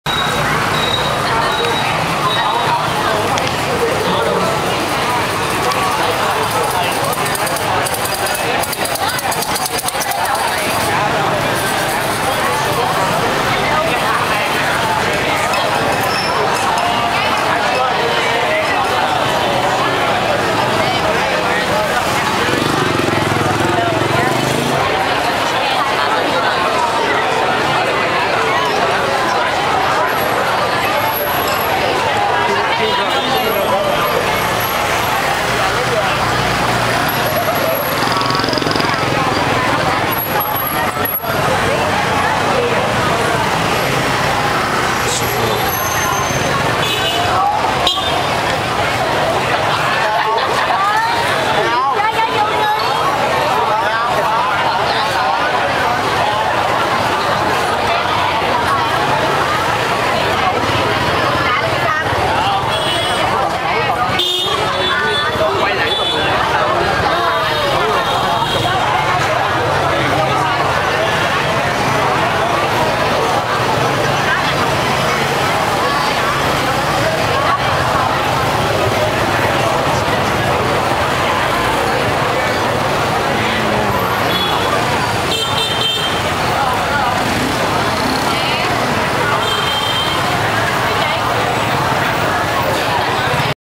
【ベトナムのカオサン通り】"BIA HOI" Open Air Vietnam Beer Bar Street,Ho Chi Minh City, Vietnam
Video of Bui Vien street,Vietnam backpacker's gathering spot in Ho Chi Minh City.
Grocery stores owner sets tables and sheets at sunset, and open instant beer bar
"BIA HOI"

ベトナム貧乏旅行者の聖地、ブイヴィエン通りの映像です。
日が落ちると、雑貨屋等の商店が軒先にテーブルとイスを並べ
即席オープンバー【ビアホイ】を始めます。